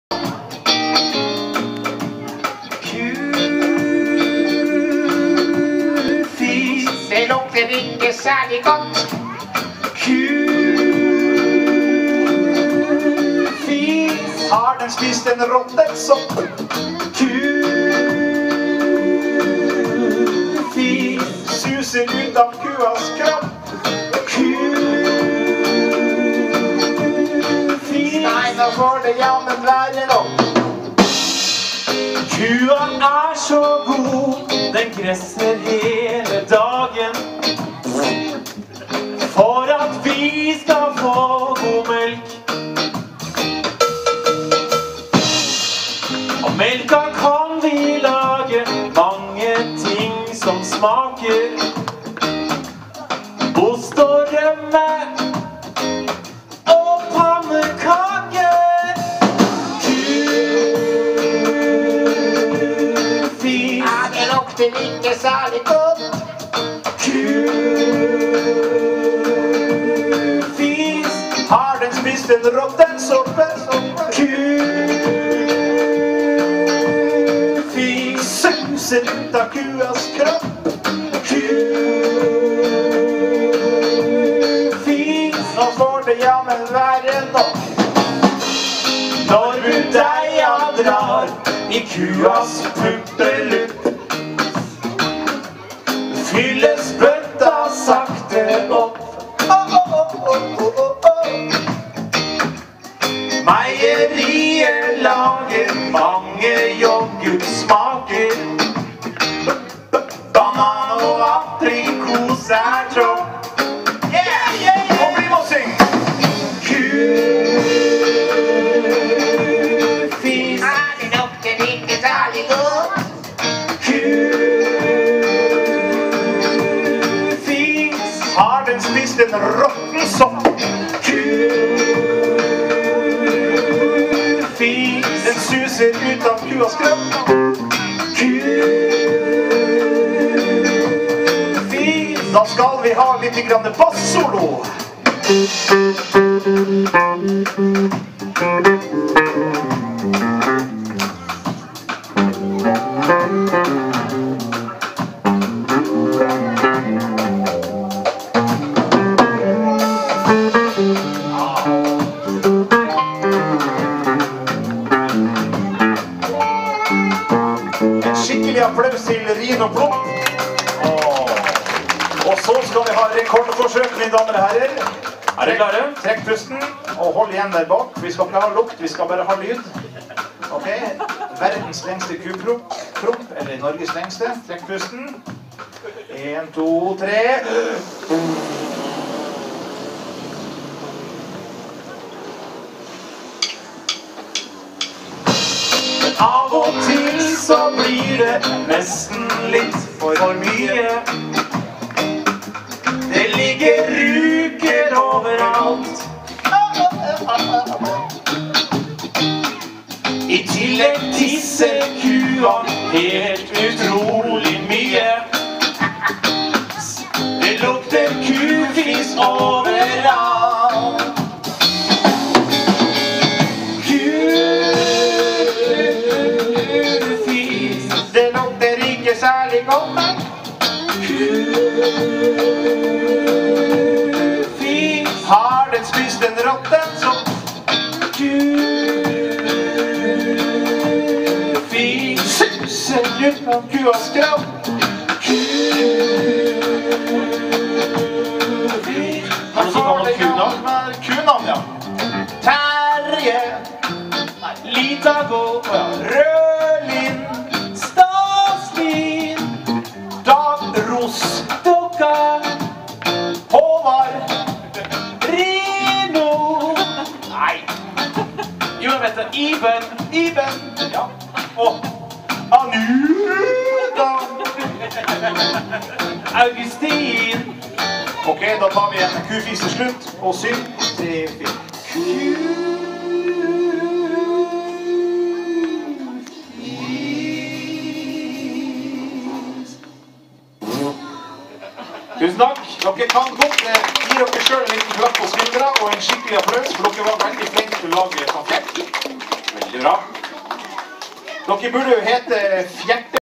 plopp_kufis_20120121.mp4
Rockebandet Plopp på Nøtterøy Kulturhus 21. januar 2012. Kufis.